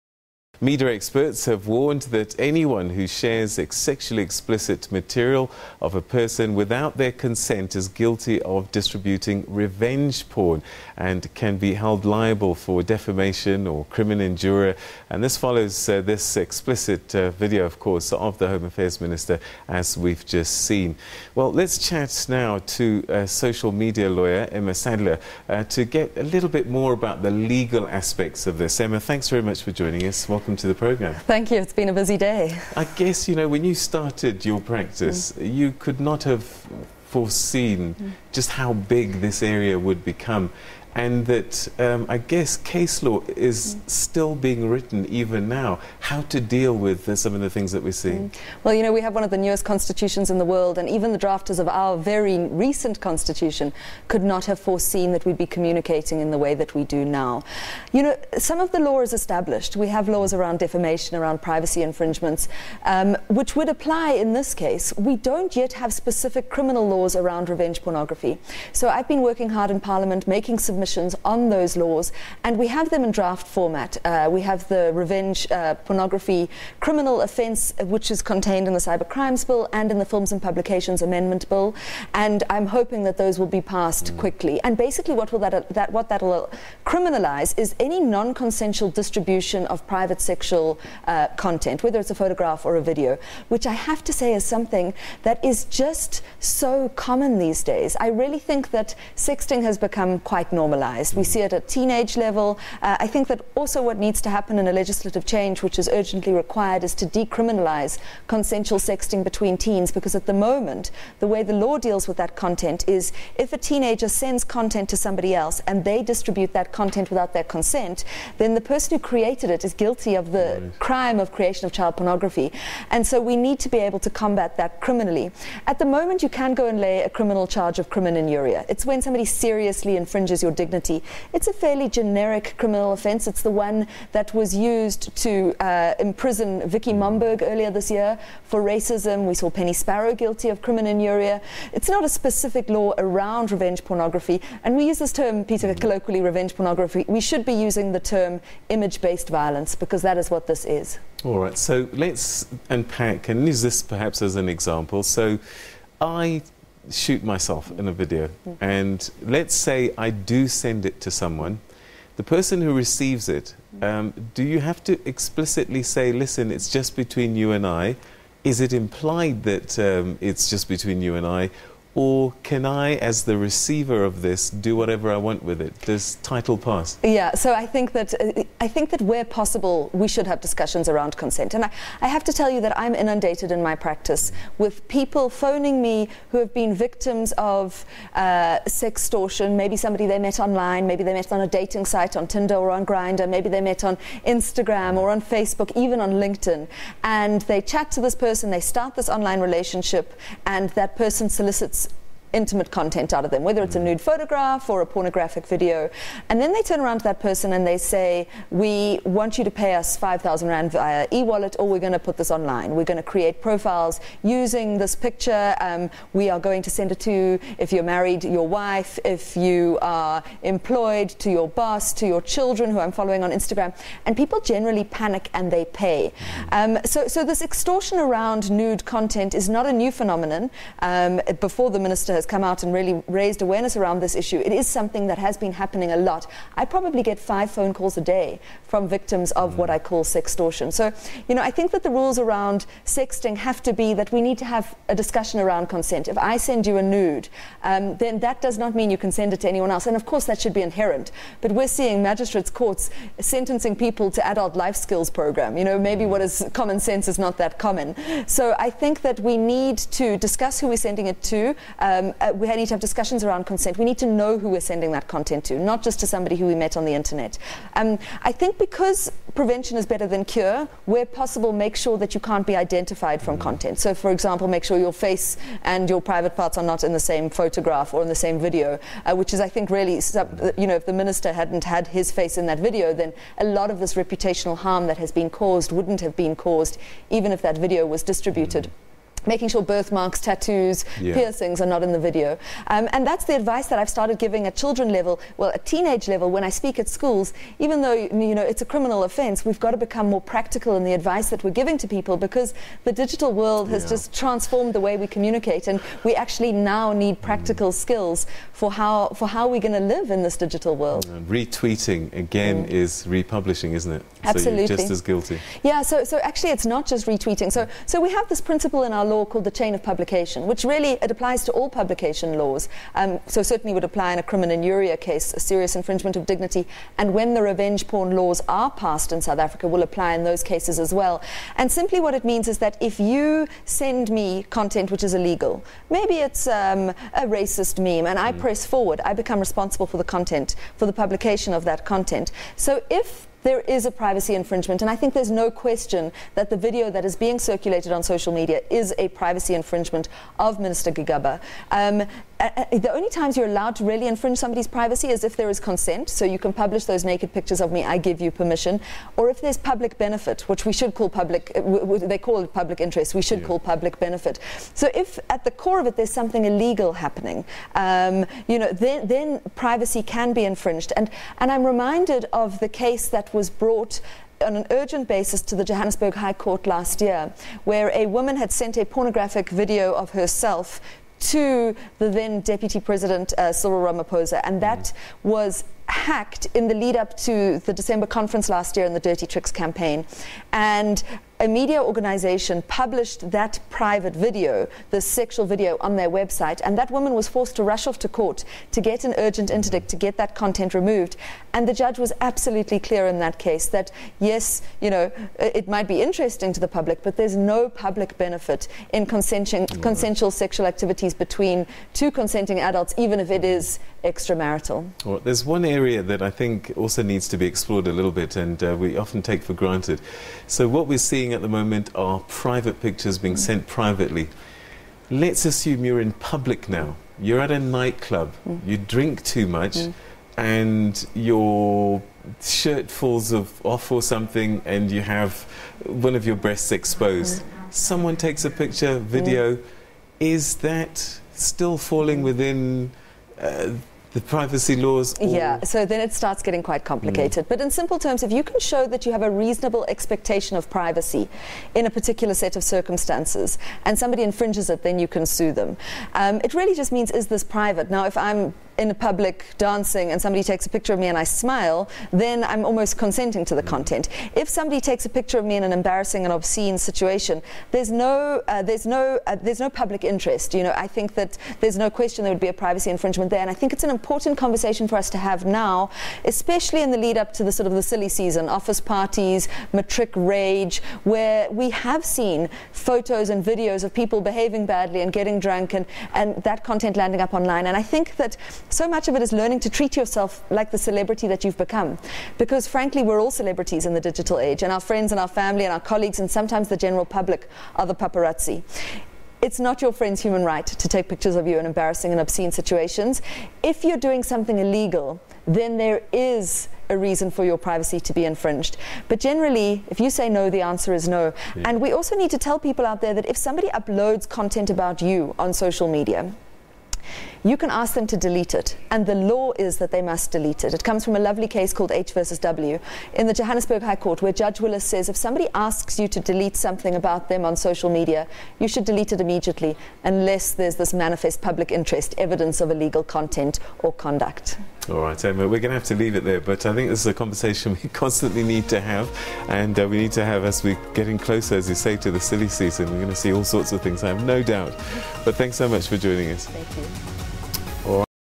Sharing of explicit content on social media: Emma Sadleir
Social media law experts have warned that anyone who shares sexually explicit material of a person without their consent is guilty of distributing revenge porn and can be held liable for defamation or crimen injuria this follows an explicit video of Home Affairs Minister Malusi Gigaba that has been doing the rounds on social media. Now to further the discussion we are joined in studio by Emma Sadleir an expert in social media law.